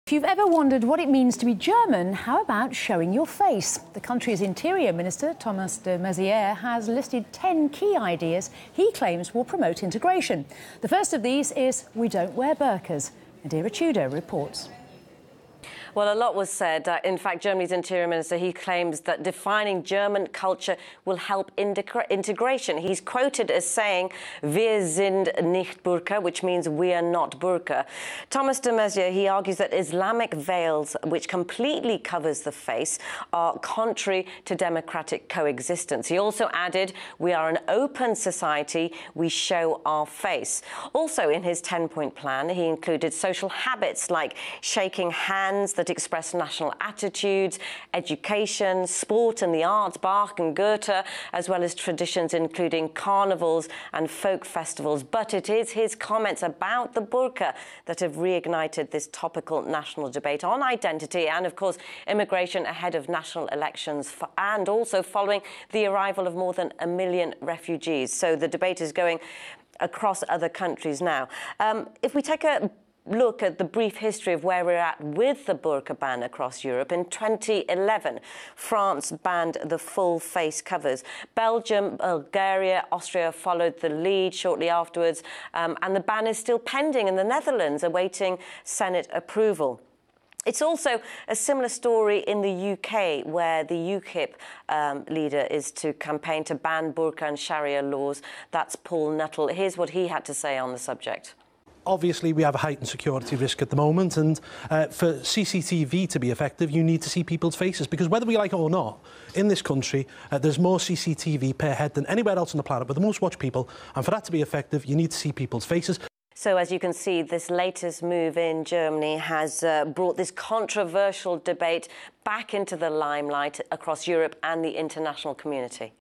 'We Are An Open Society. We Show Our Faces. We Don't Wear Burqa' - German Interior Minister
May 2, 2017 - The German Interior Minister has expressed support for the idea of introducing a “dominant culture” for German society that would define public life and serve as a guideline for the integration of migrants.